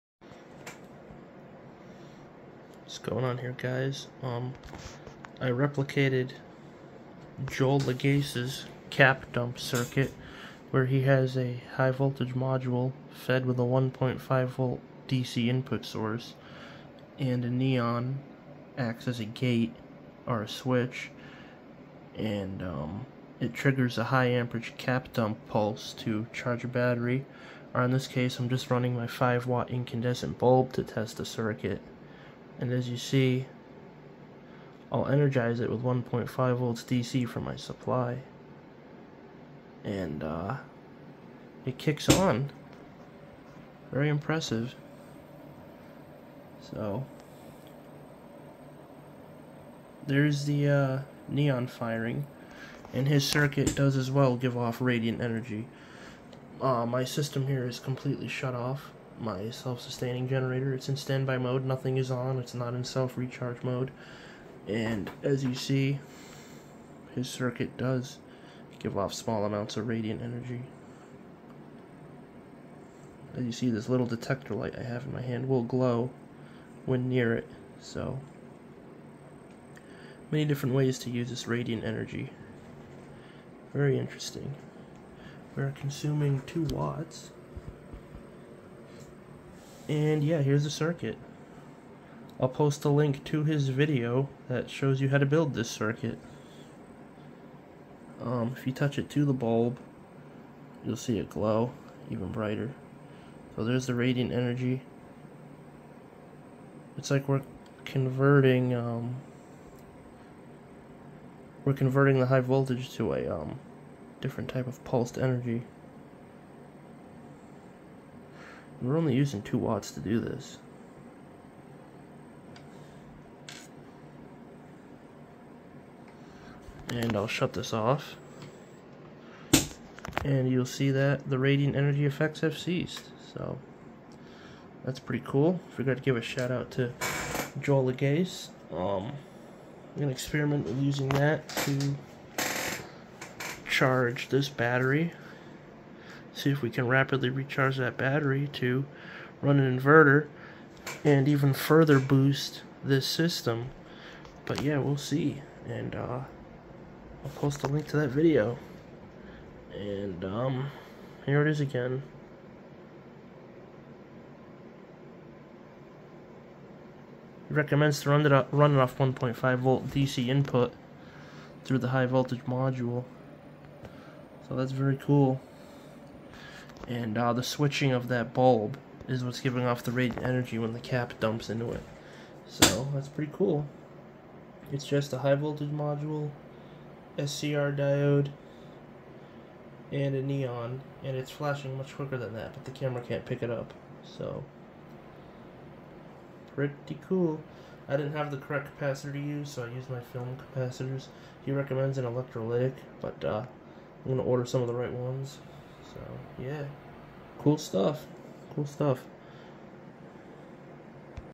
🌟Replicating Joel's Radiant Energy Amps Per Second Cap Dump Circuit To Rapidly Charge Batteries🌟
🌟 Link To My PDF On How To Build These Transmitters & Systems

🌟 Joel's Channel

🌟 Joel's Cap Dump Circuit Video

🌟 Joel's 100 Watts Of Free Energy Circuit Via Cap Dumping

In This Video, I Replicate & Confirm Joel's Amps Per Second Radiant Energy Cap Dump Circuit. I'm Probably Going To Experiment With It To Further Amplify My Self Sustaining Wireless Power Generator & A Huge Thank You & Shoutout To Joel Lagace.